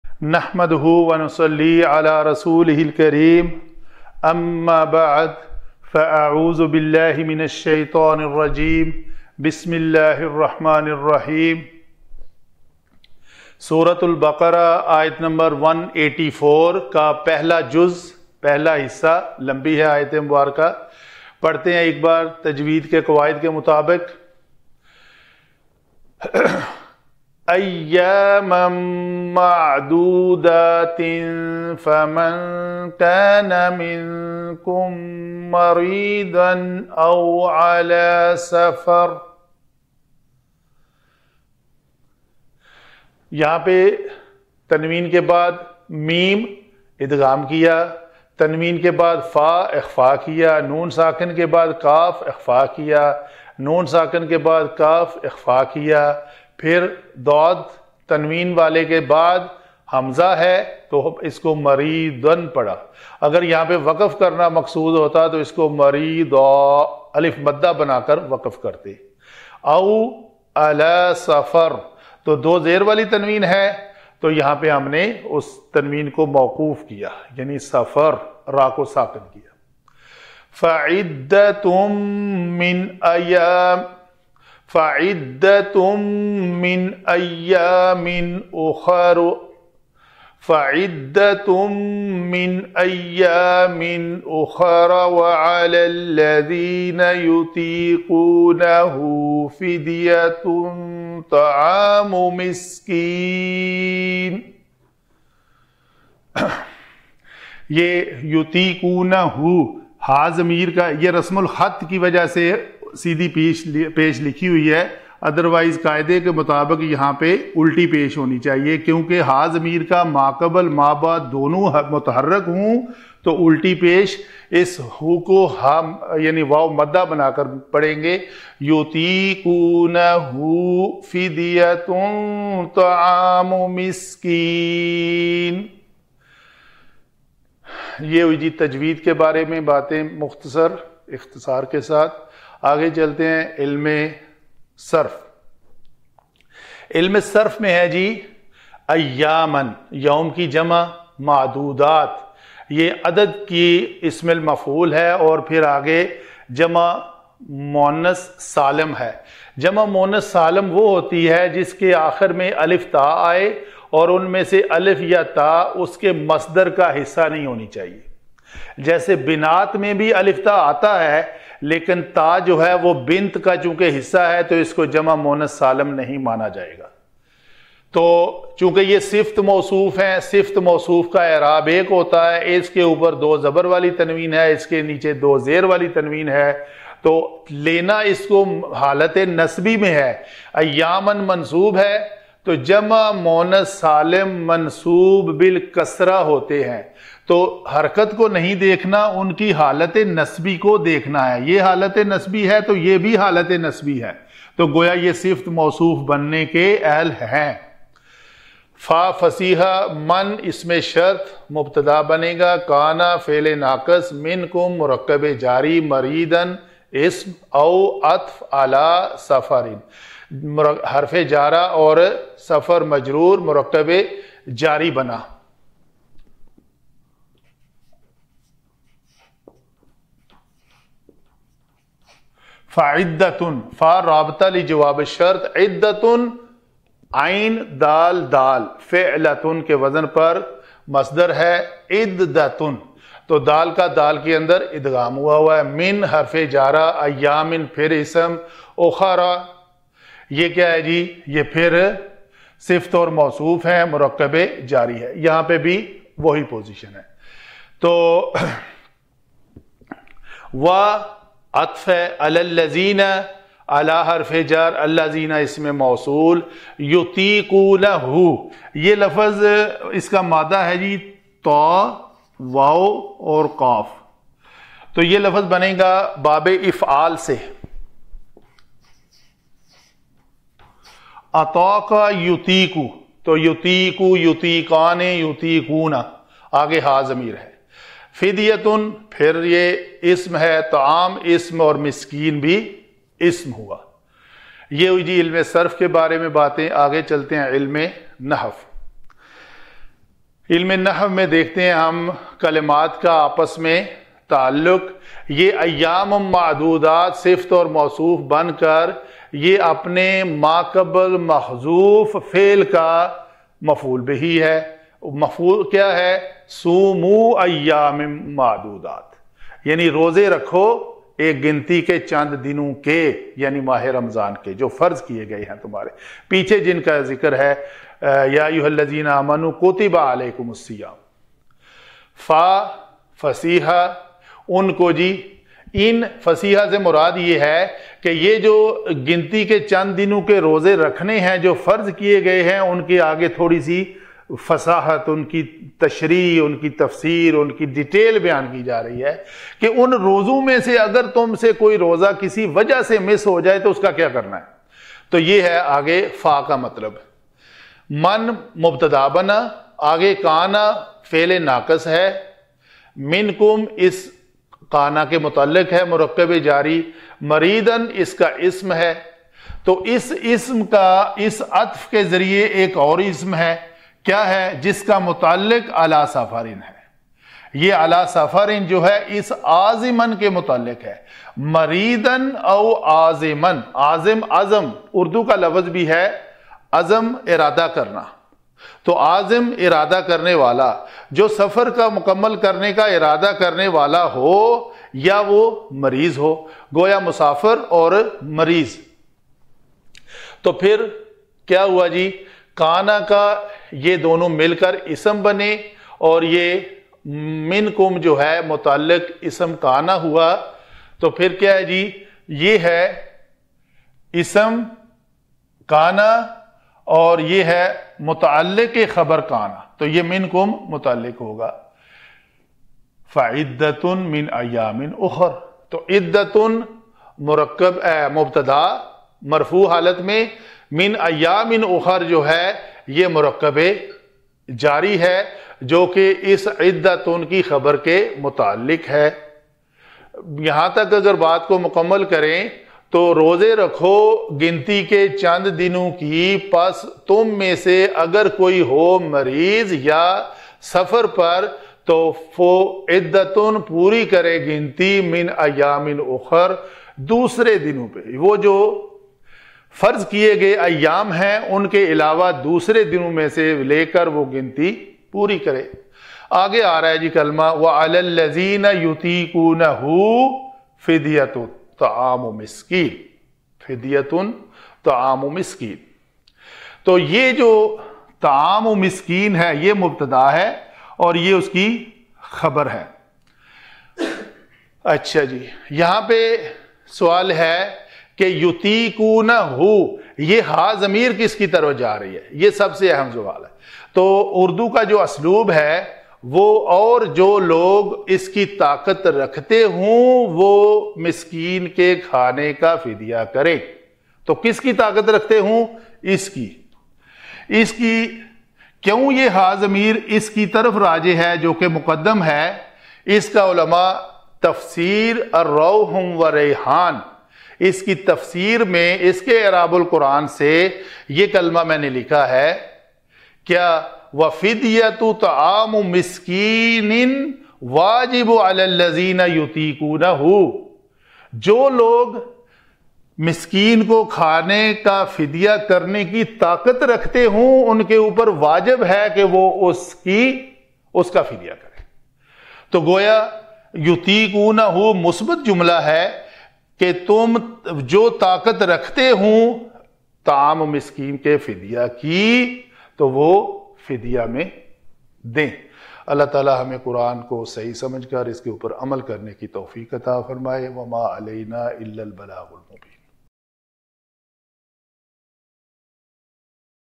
Arabic Grammar: Surah Al Baqarah, Ayah 184-A (02 : 184-A) (URDU)
Arabic Grammar Course for Understanding Qur'an, By: Muhammad Naveed Iqbal.
Surah Al Baqarah, Ayah 184-A (02 : 184-A) (URDU)